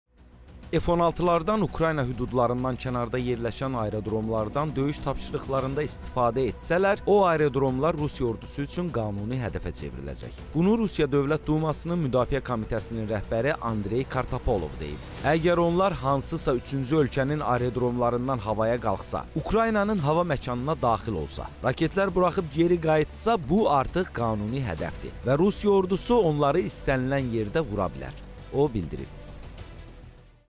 Rusiya bu hədəfi 3-cü ölkədə də vuracaq | Kartapolovdan vacib açıqlama
“F-16”lardan Ukrayna hüdudlarından kənarda yerləşən aerodromlardan döyüş tapşırıqlarında istifadə etsələr, o aerodromlar Rusiya Ordusu üçün qanuni hədəfə çevriləcək.
Bunu Rusiya Dövlət Dumasının Müdafiə Komitəsinin rəhbəri Andrey Kartapolov deyib.
“Əgər onlar hansısa üçüncü ölkənin aerodromlarından havaya qalxsa, Ukraynanın hava məkanına daxil olsa, raketlər buraxıb geri qayıtsa, bu, artıq qanuni hədəfdir və Rusiya Ordusu onları istənilən yerdə vura bilər”, – o bildirib.